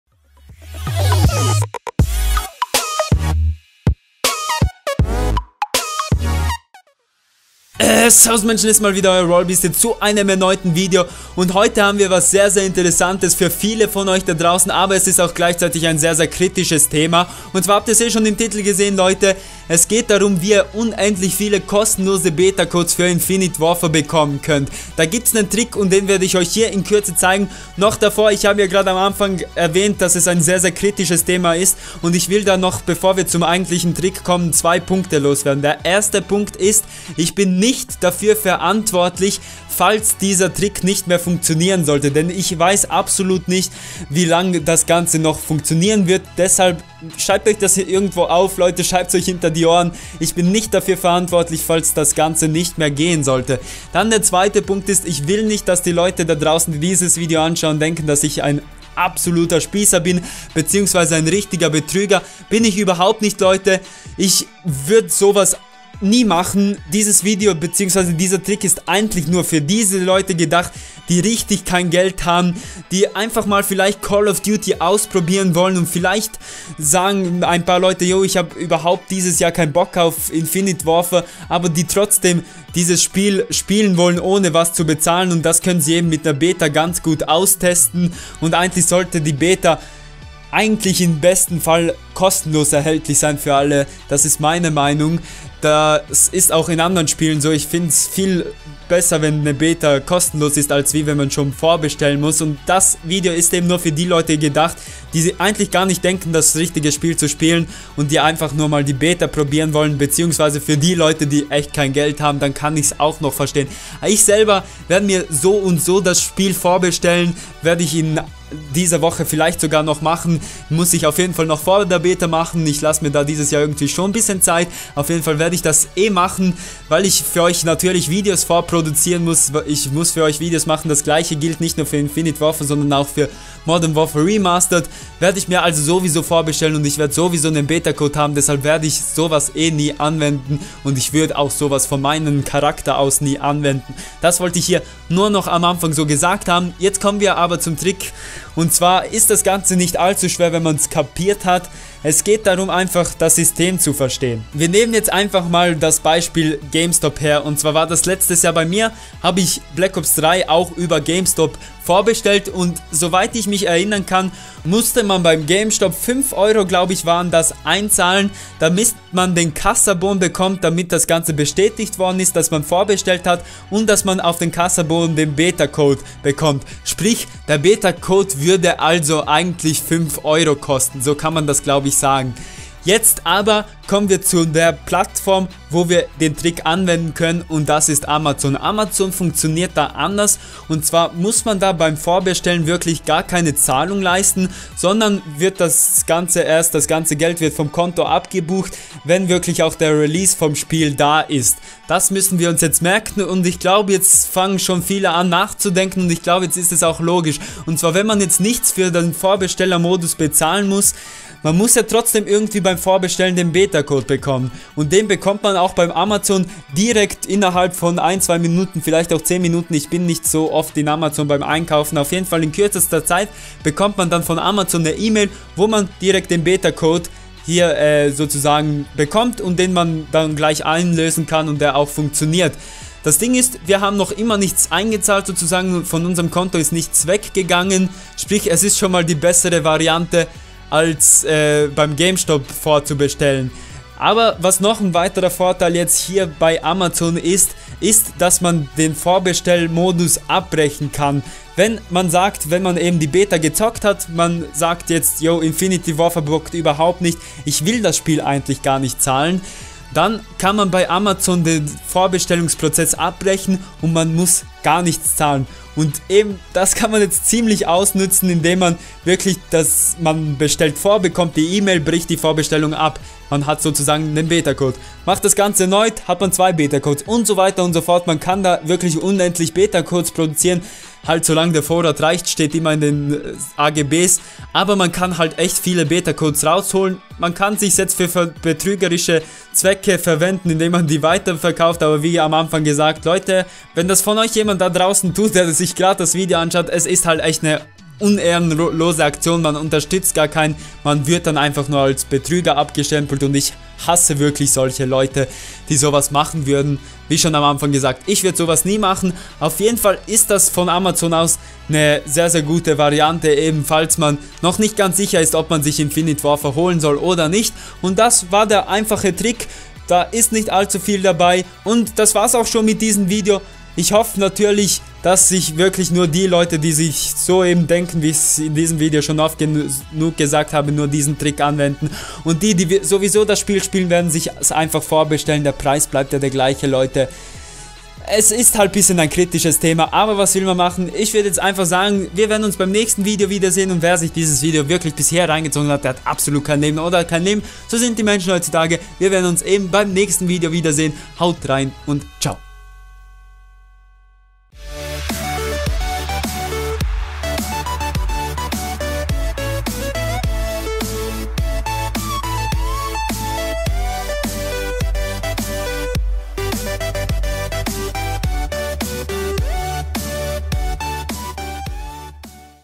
UNENDLICH BETA CODES! KOSTENLOSE IW BETA CODES! - Infinite Warfare [German]
♛Willst du wissen wie man kostenlose Beta Codes für IW bekommt?! :o
▼Öffne mich, sonst mag ich dich nicht mehr!▼

♛In diesem Video zeige ich, wie man unendlich viel Beta Codes für Infinite Warfare bekommen kann, und das ganze sogar kostenlos!

▼♛Royal-Links♛▼

✖Skype:♢Royal.Beastz

♛Hat es dir gefallen??? Zeig es mit einem Like!!! ◠ᴥ◠